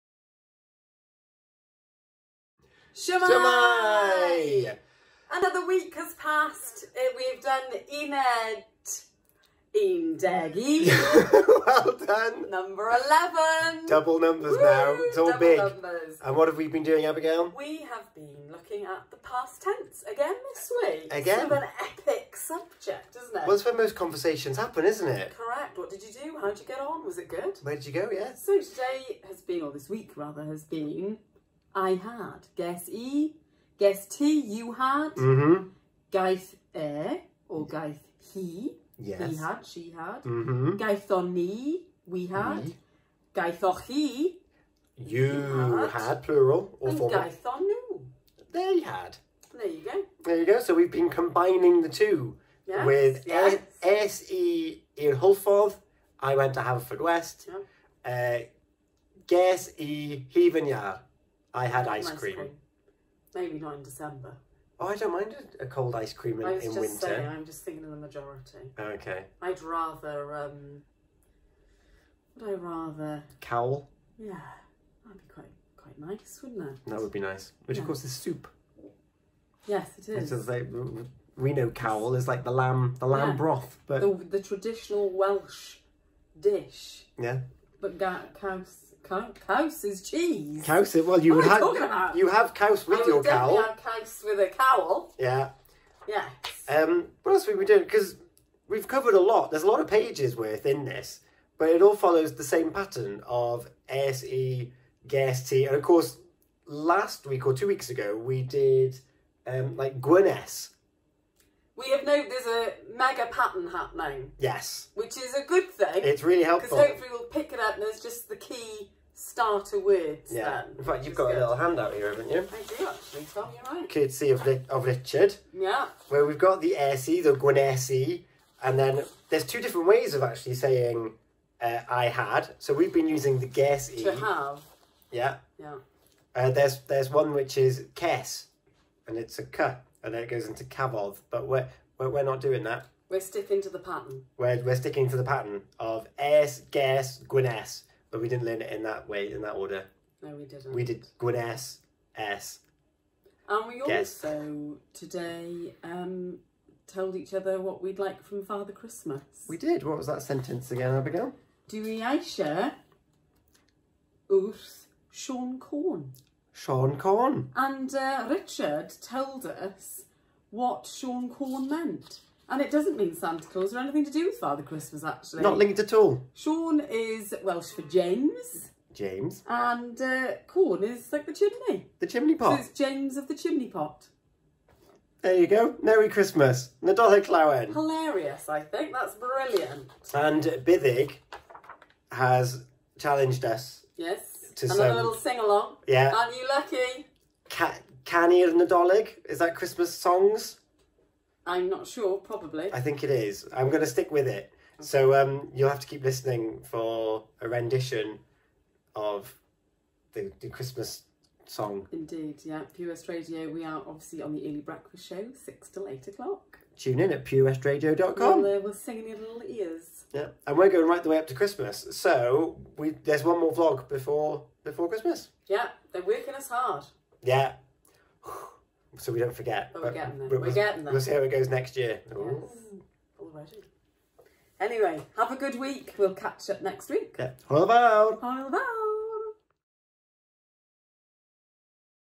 Learn Welsh Week 11 - I had!
Shwmae! Welcome back to another week of Welsh learning!

We are continuing our theme of learning about the past tense and bringing conversations to life! This week we focussed on 'I had' - 'ges i'.

It feels very good to be able to have the tools to go forth and think of conversations with other people, even if they are still in our heads a little at this moment in time, ond, it is only our 11th week of learning! We've done so much in such a short period of time.

The tutors, Buddug a Richard are excellent - Ardderchog! Gwych! Bendigedig! As are the rest of the classes.

Learn Welsh Pembrokeshire has some more classes starting in January! Why not take up a new language?!

Tom a Abz!